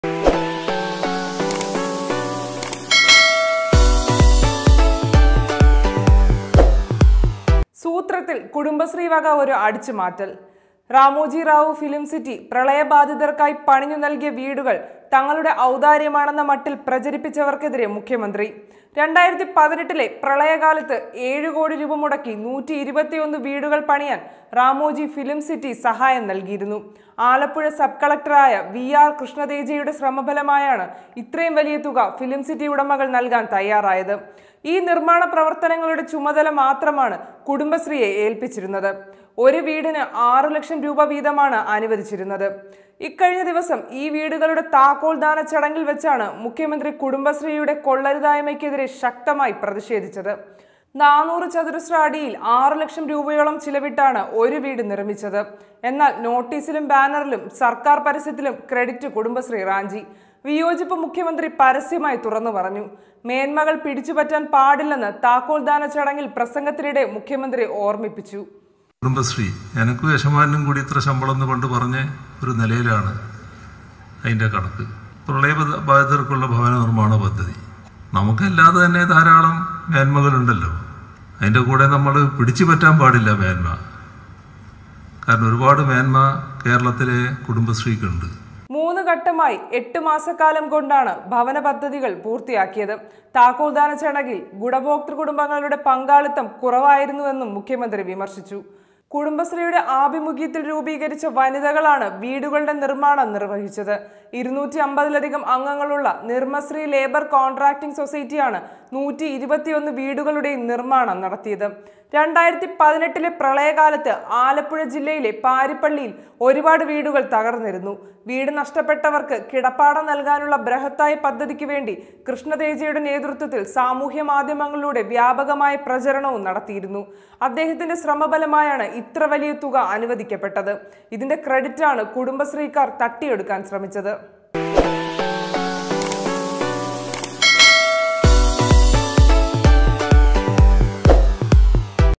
ക്രഡിറ്റ് അടിച്ച് മാറ്റാൻ നോക്കിയ കുടുംബശ്രീയെ പൂട്ടി മുഖ്യമന്ത്രി
കുടുംബശ്രീയുടെ അടിച്ചുമാറ്റല്‍ മുഖ്യമന്ത്രി കൈയോടെ പിടിച്ചു; റാമോജി റാവു ഫിലിം സിറ്റി നല്‍കിയ ഏഴു കോടിയുടെ ക്രെഡിറ്റ് സ്വന്തം അക്കൗണ്ടിലാക്കി; നന്ദിക്കേട് അനുവദിക്കാനാവില്ലെന്ന് മുഖ്യമന്ത്രി